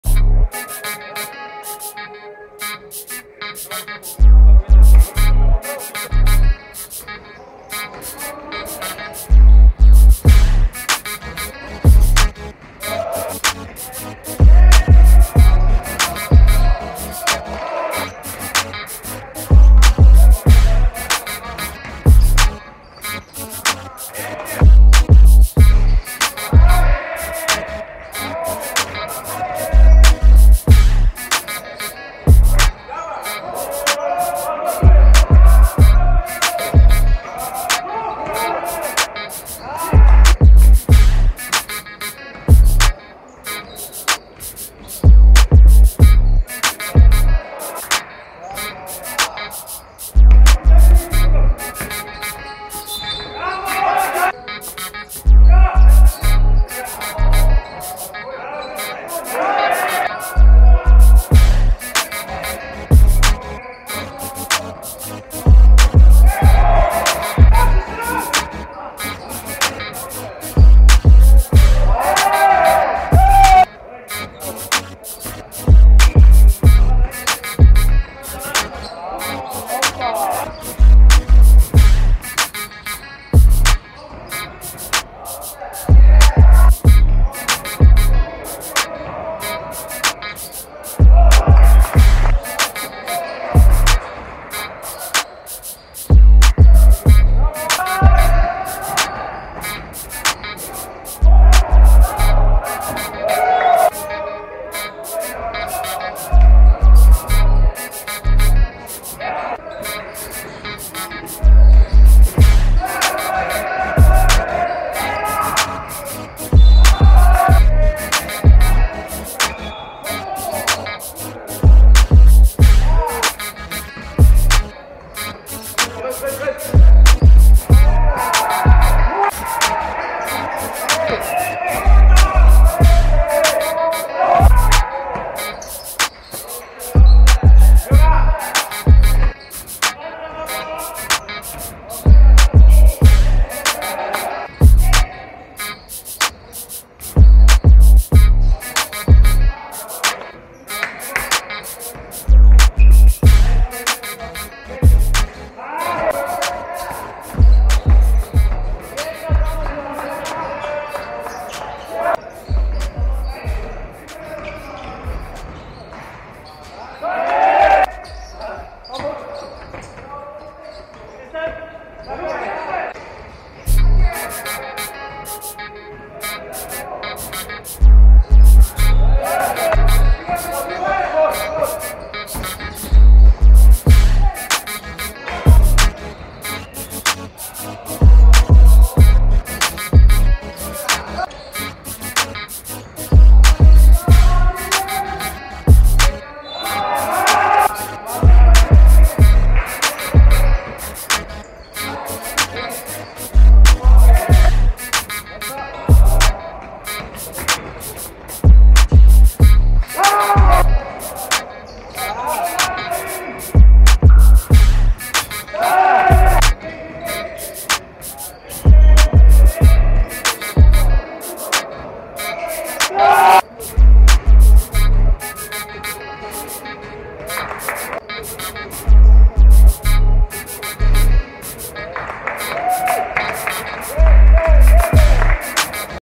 29 03 Nola vs Seattle – Draft Liga Środa D2 Wiosna 2023
NOLA 52:45 SEATTLE
V.Demchuk 12pkt 5zb 5blk
K.Górecki 13pkt 6zb